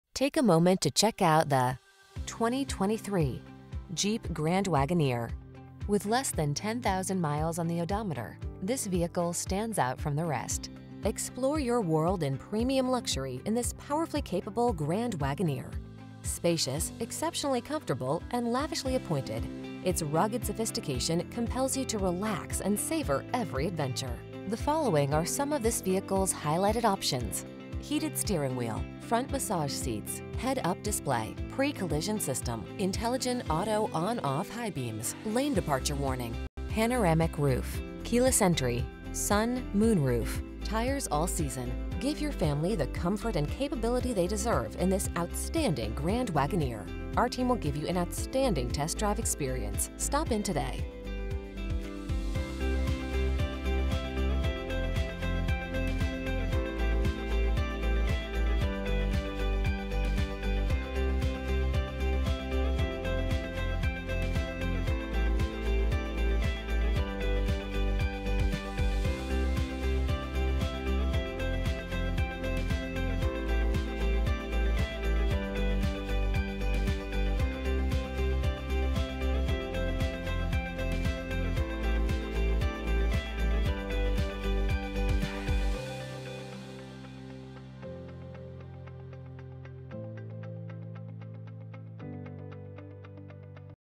2023 Jeep Grand_Wagoneer Series III CO Denver, Arvada, Lakewood, Bloomfield, Thorton
Pre-Owned 2023 Jeep Grand_Wagoneer Series III available at Phil Long Ford of Denver located in 7887 West Tufts Avenue, Denver, CO, 80123. Servicing the Denver, Arvada, Lakewood, Bloomfield, Thorton area.
Please mention the stock A30960A when you contact the dealership for this vehicle.